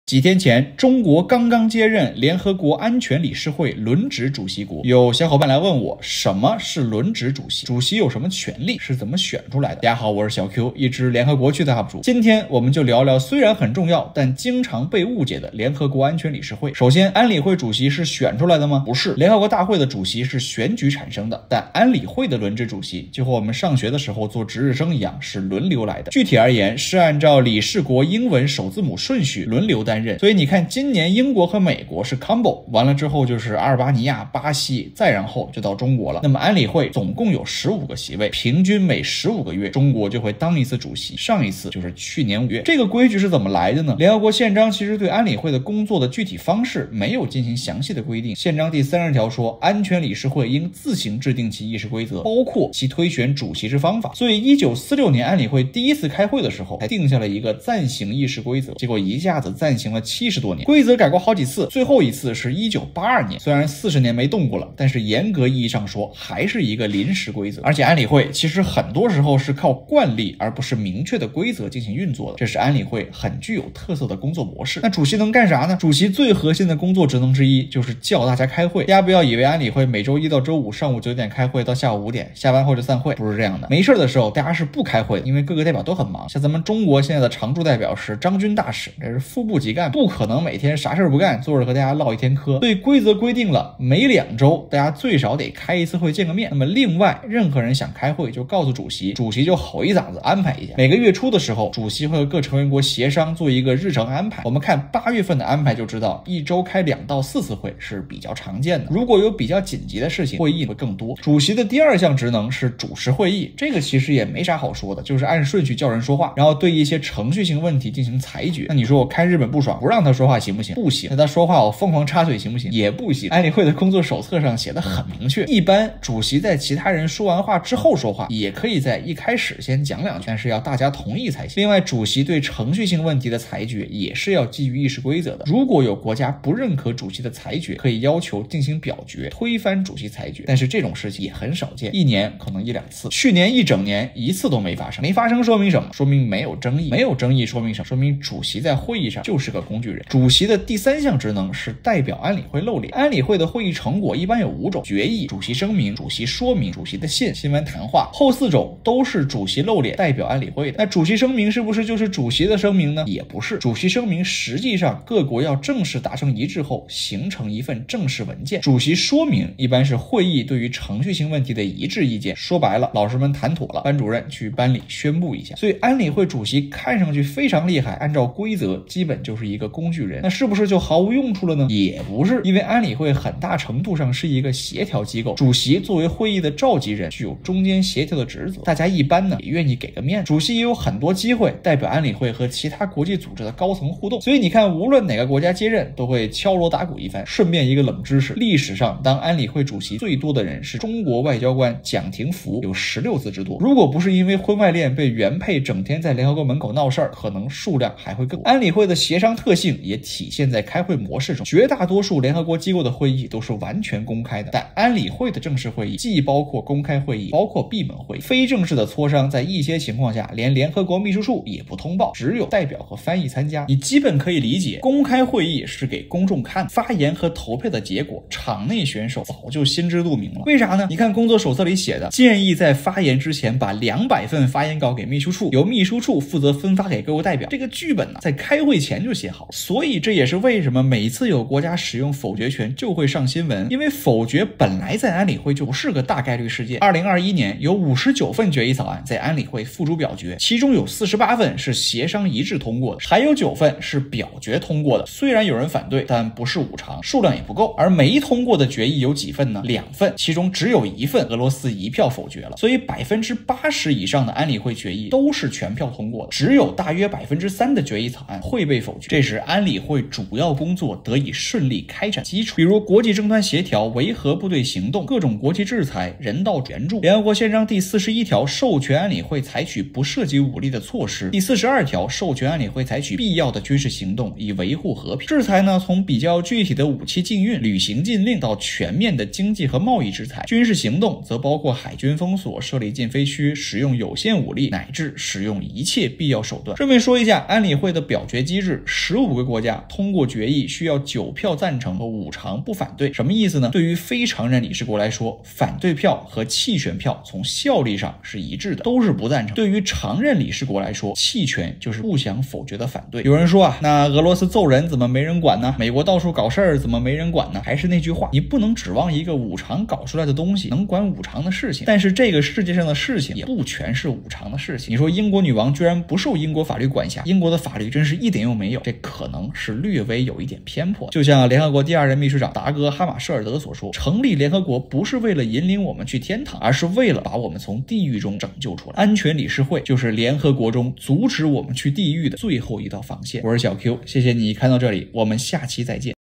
联合国安理会主席能管什么？中国人创联合国安理会主席就任最多次历史纪录，却因婚外恋被迫下岗！｜小Q解读联合国
2021年，有59份决议草案在安理会付诸表决，其中有48份协商一致通过，9份表决通过，仅有2份未获通过，仅有1份被俄罗斯一票否决——然而在新闻里，似乎安理会的主要任务就是一票否决，这可能是一种常见的误解。

小Q简介：
新南威尔士大学社会科学博士候选人
国际关系研究者，专注联合国、国际法和全球治理研究。
致力于推动2030可持续发展目标的实现。